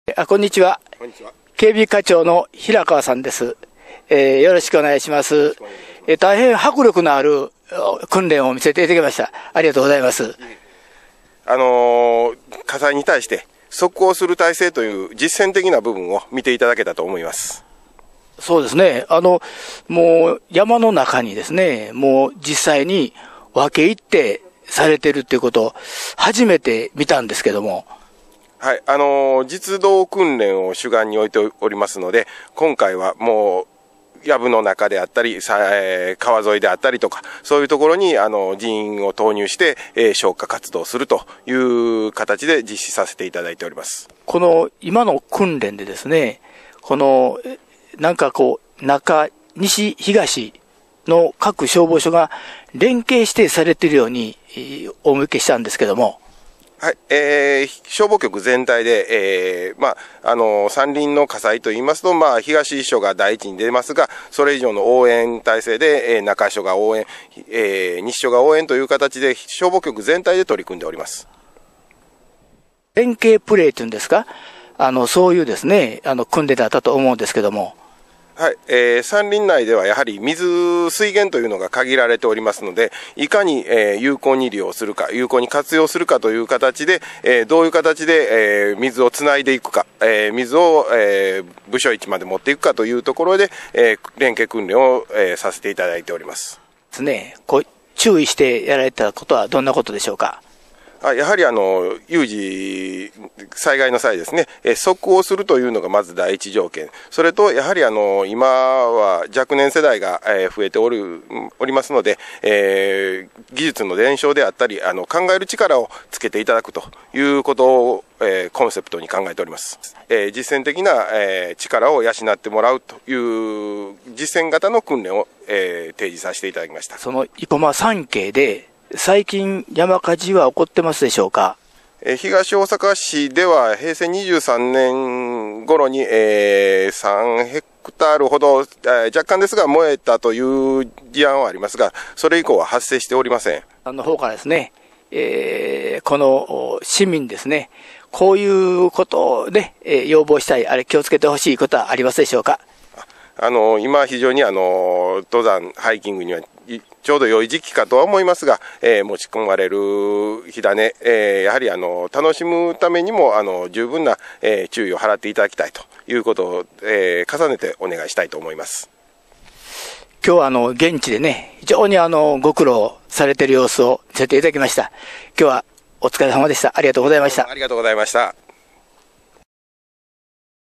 林野火災訓練 インタビュー
生駒山の林野火災に備え、実動訓練がおこなわれた。現場からのインタビュー　　どーなってる東大阪